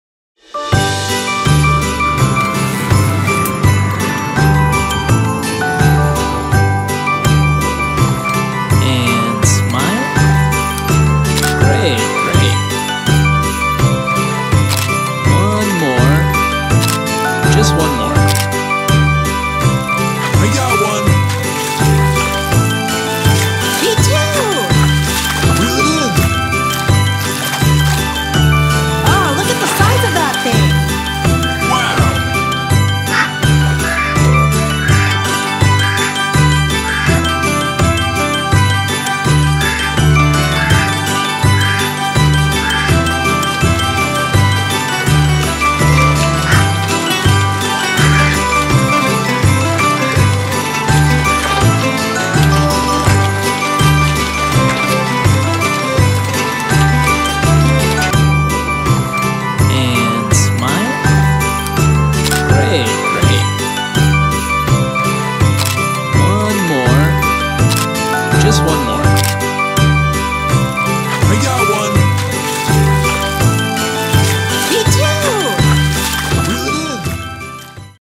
Lemax Birch Creek Ice Fishing Festival SKU: 25862
It's that time of year again and the Birch Creek Ice Fishing Festival is underway. The fishermen and fisherwomen are bundled up and ready with their poles to land the biggest catch of the day. The Tackle Shop is festively lit for the occasion making this annual event a favorite in town for participants and spectators alike.